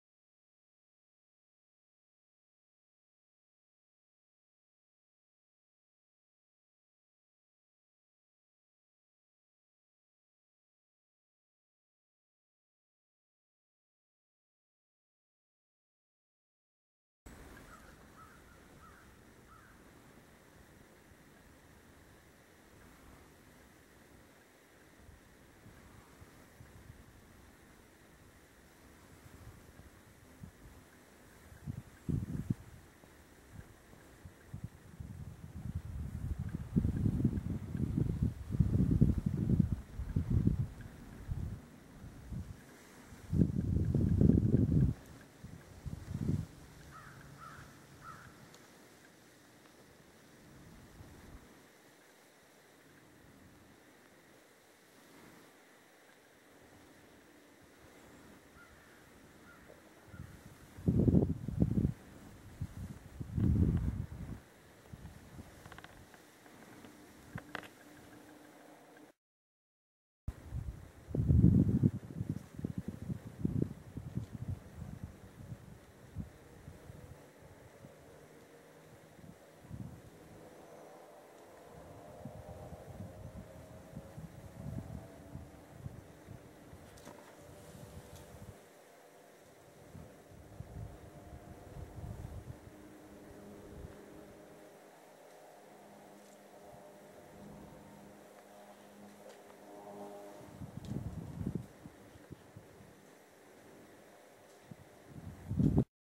Kingdom Come State Park in Cumberland, Kentucky: 12 O'Clock Overlook and Creech Overlook
A short video showing the great views of the Kentucky Plateau as viewed from the 12 O'Clock and Creech Overlooks in Kingdom Come State Park on Pine Mountain in Cumberland, Kentucky.